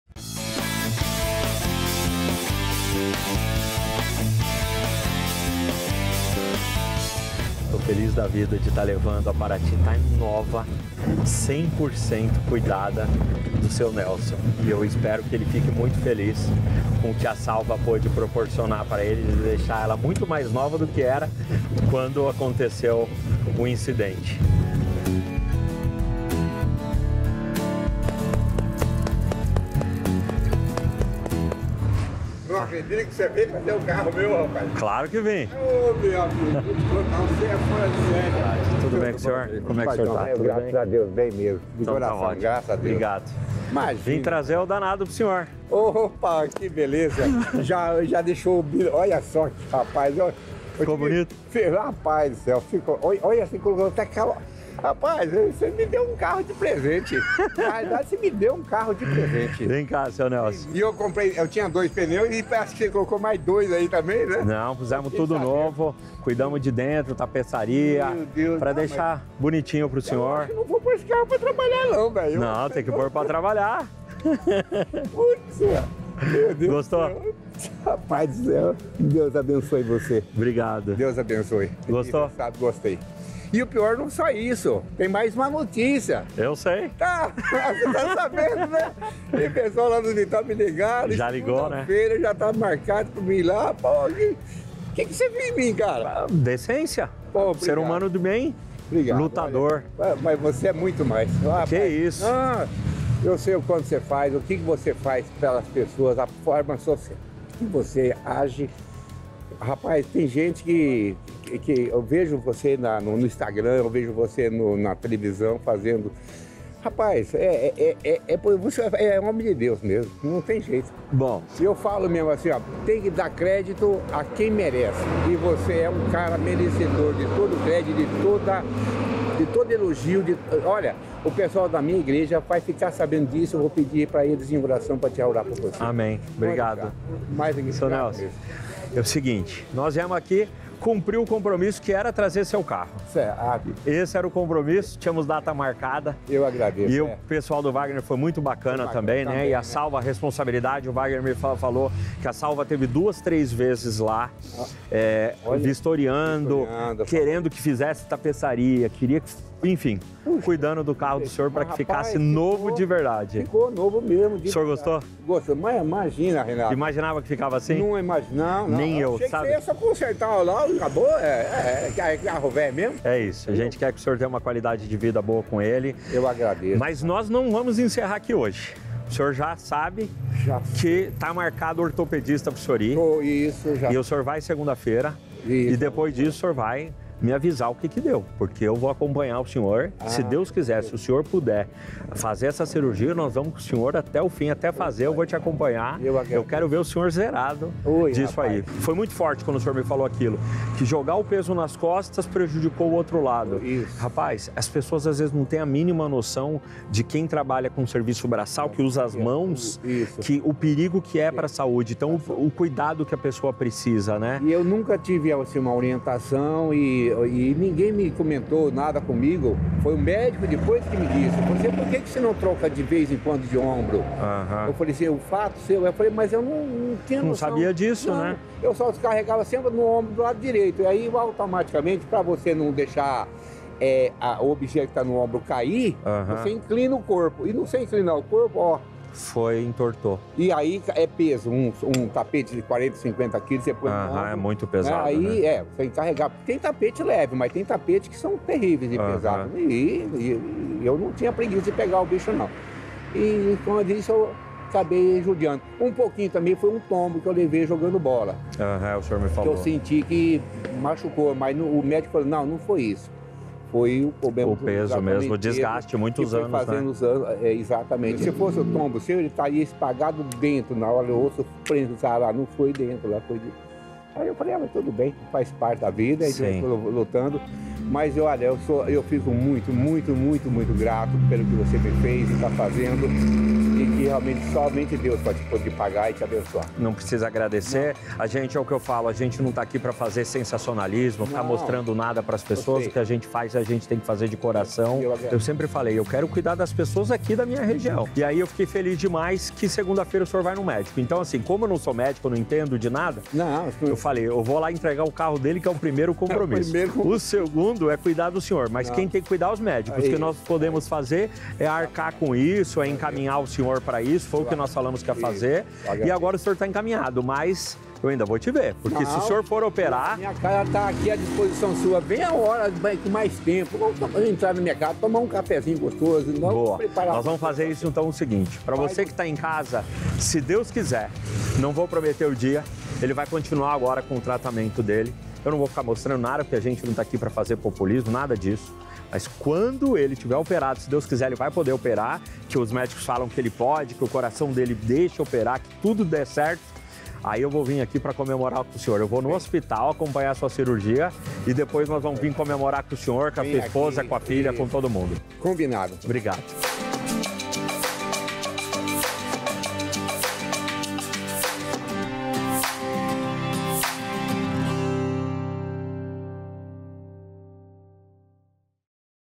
VALE SORTE - SALVA - ENTREGA PARATI (PARTE 2)
Agora com a Paranati renovada, o Renato vai até a casa do Sr. Nelson para devolver o carro impecável, como ele havia prometido e ainda teve mais uma surpresa...
Enfim agora o Nelson conseguirá realizar o tratamento com um Ortopedista e recuperar sua qualidade de vida!
Que abençoe o senhor, nós estaremos acompanhando esse processo e ficaremos na torcida para que tudo dê certo!
Confira no vídeo como foi essa linda entrega!